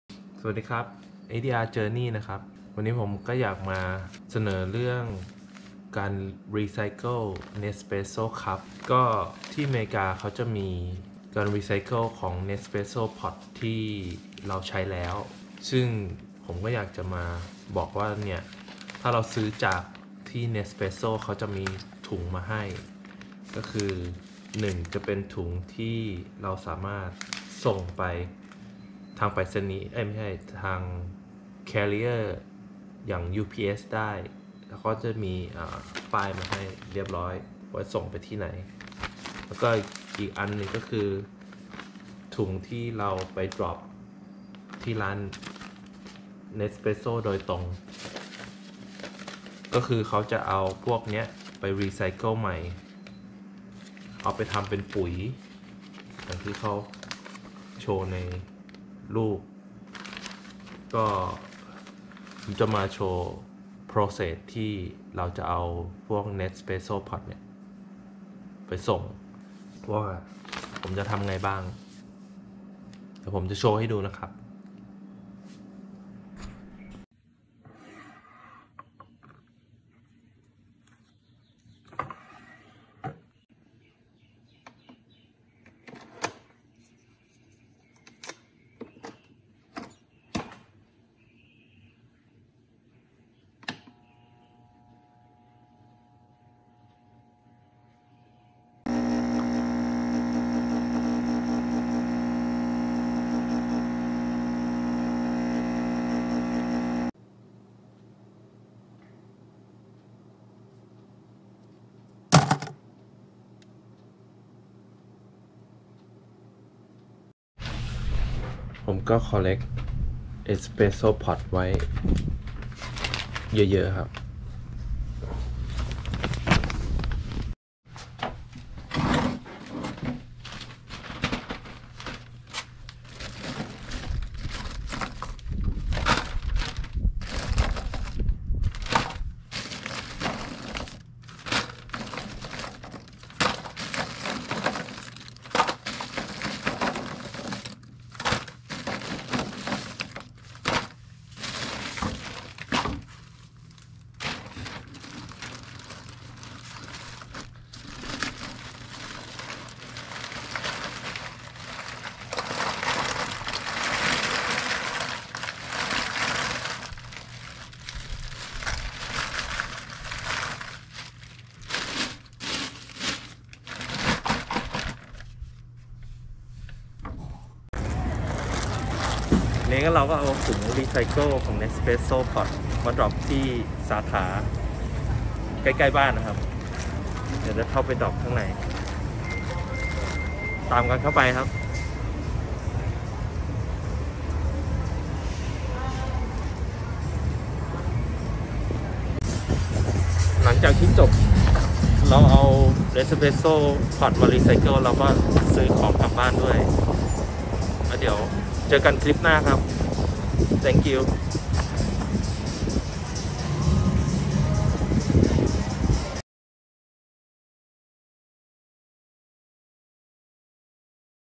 How to recycle Nespresso pods (ASMR) รีไซร์เคิล Nespresso แคปซูล ได้อย่างไร
สำหรับคนที่ชอบดื่ม Nespresso หลังจากเราทำกาแฟแล้ว ตัวแคปซูล ไปส่งรีไซร์เคิลที่ไหน ผมทำให้ดู